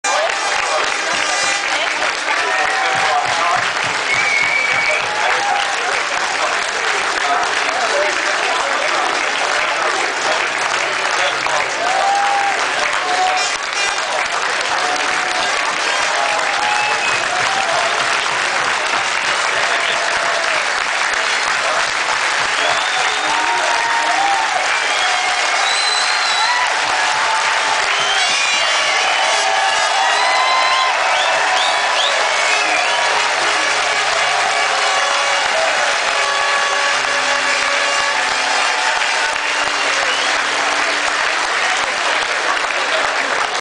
Lewis Hamilton UK GP Pole Position standing ovation
The standing ovation given the Lewis Hamilton as he comes through the end of the lap after getting the Pole Position at the 2007 British Formula 1 qualifying.